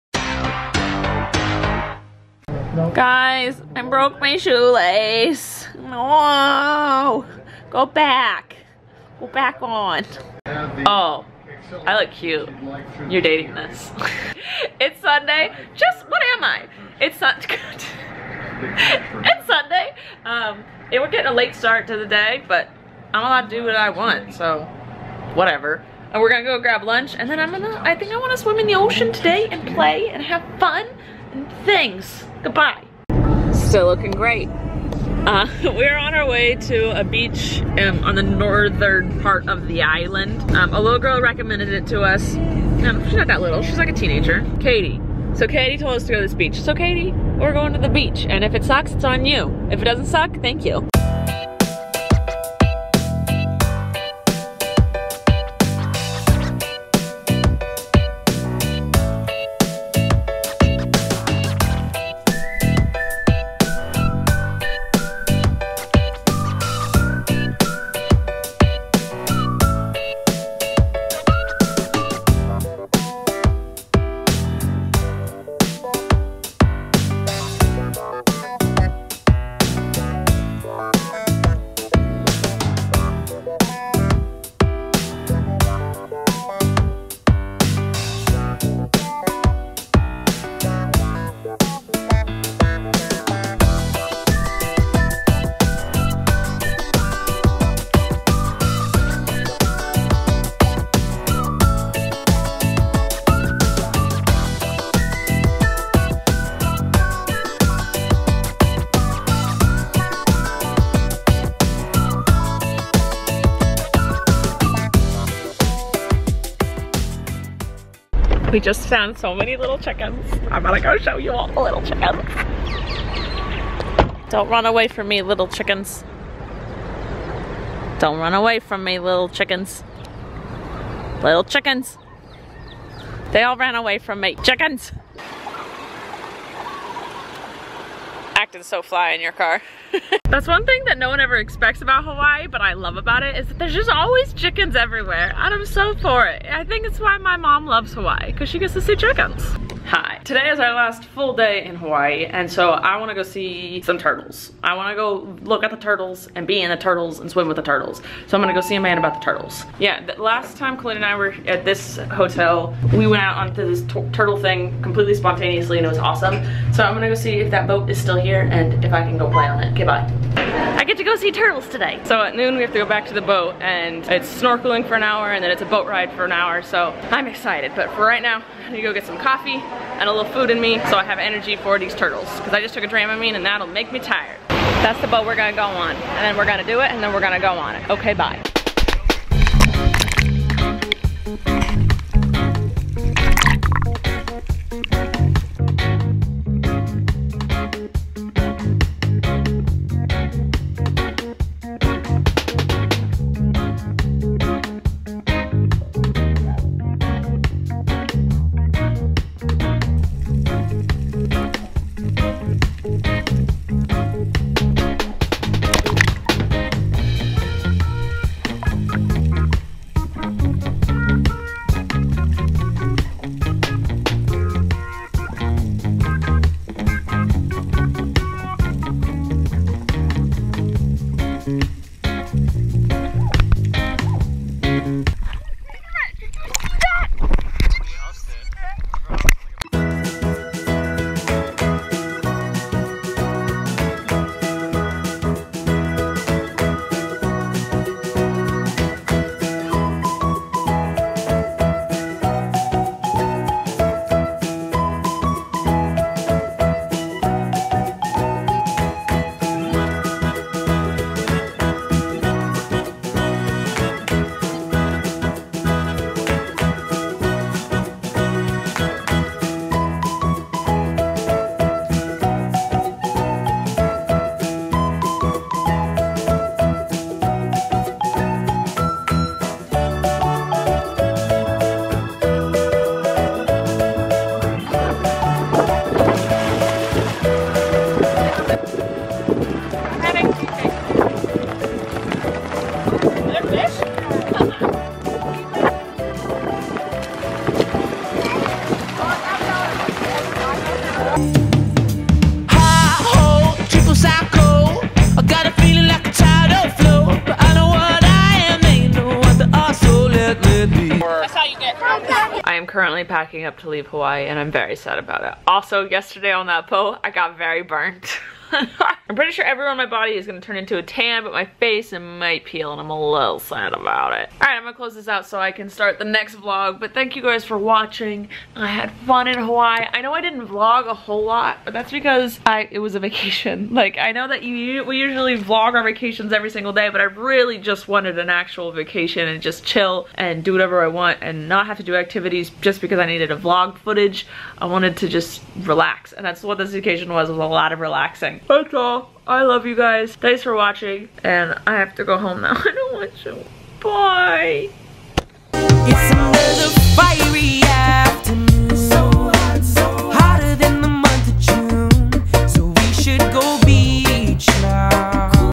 SWIMMING WITH TURTLES!
I had so much fun in Hawaii. I mean, who wouldn't?

Rachel Ballinger
Just make sure you SEND WITH A STAMPED SELF-ADDRESSED ENVELOPE or else I won't be able to write you back :(

and iMovie